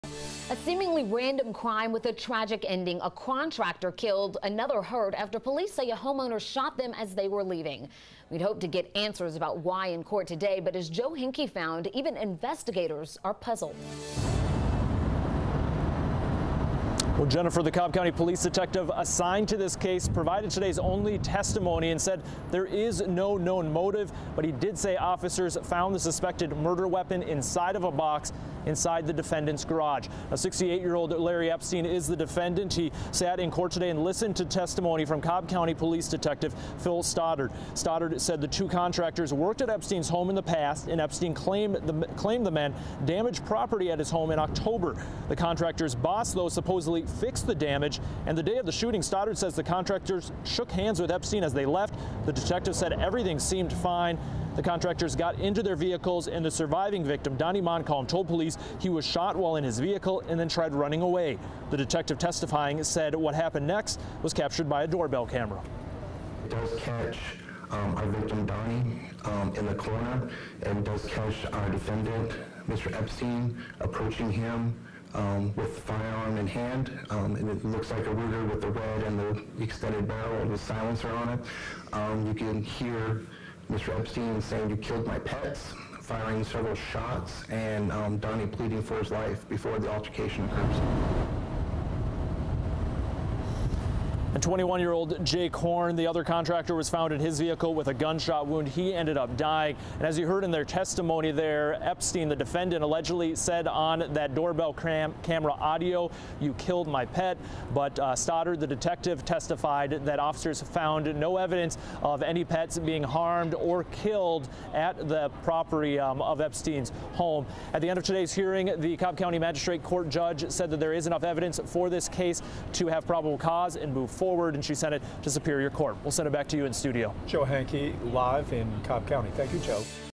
Homeowner accused of shooting contractors appears in court for preliminary hearing
A detective gave testimony